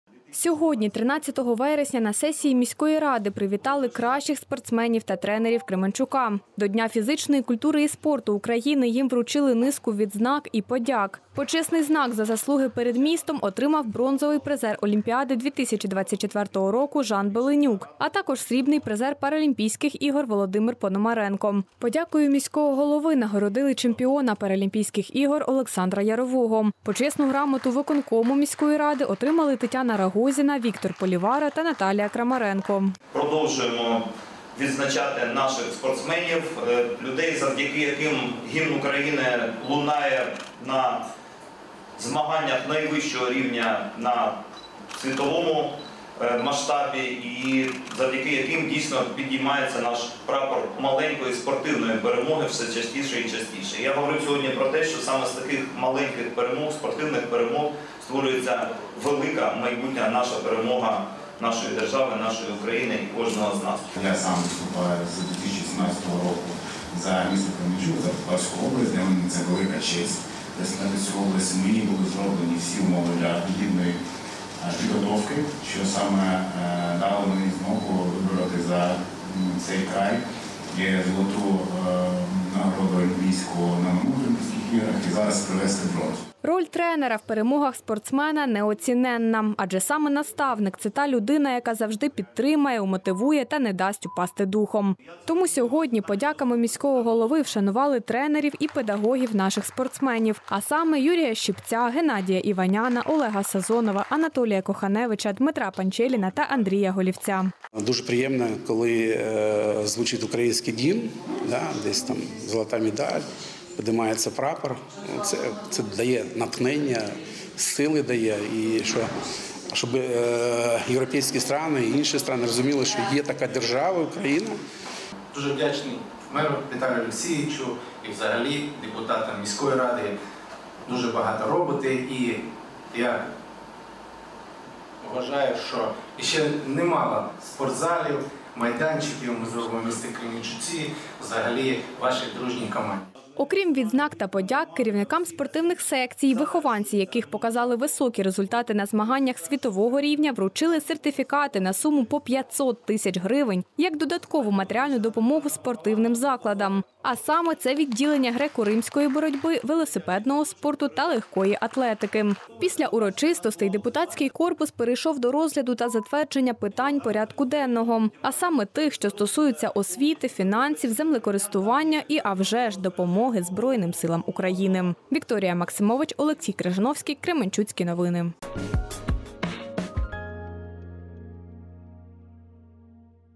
Відбулася сесія Кременчуцької міської ради, на якій привітали найкращих спортсменів, тренерів міста
Відбулася сесія Кременчуцької міської ради, на якій привітали найкращих спортсменів та тренерів міста